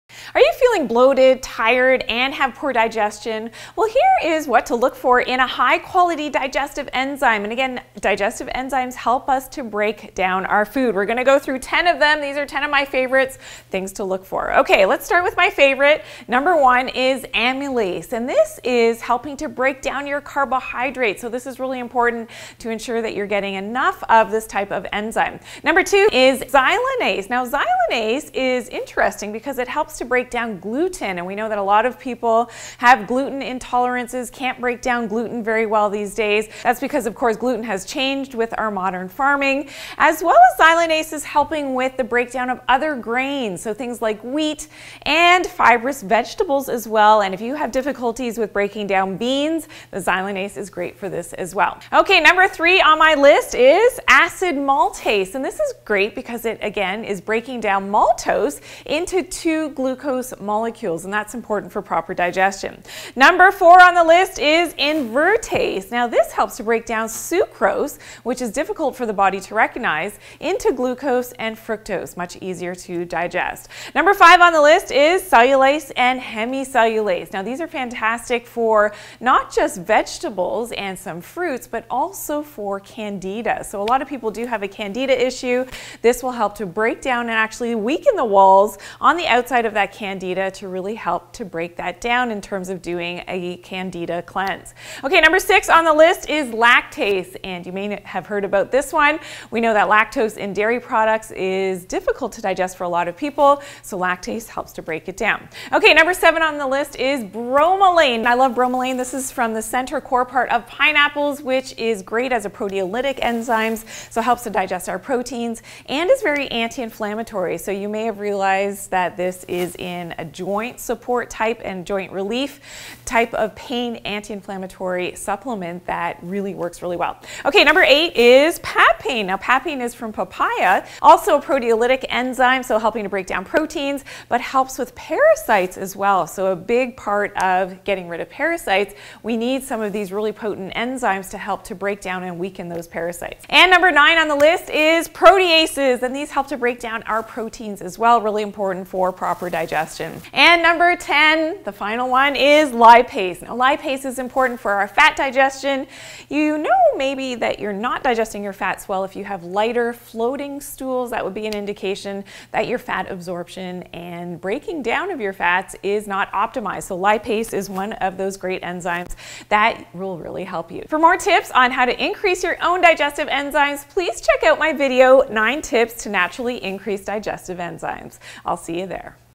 What to Look for in a High-Quality Digestive Enzyme | Dr. Janine
Dr. Janine shares what to look for in a high-quality digestive enzyme.  She talks about how amylase will help to break down carbs and starches.  She looks at how xylanase is great for breaking down gluten and fibrous veggies.  Dr. Janine explains how acid maltase helps break down maltose.  She talks about how invertase helps digest carbs.  She looks at how cellulase and hemicellulase are not only good for breaking down carbs and veggies but candida too.  Dr. Janine talks about how lactase helps break down the lactose in dairy products.  She looks at bromelain, papain, and proteases for the breakdown of proteins.  Lastly, Dr. Janine explains how lipase is excellent for breaking down fats.

J9CON230046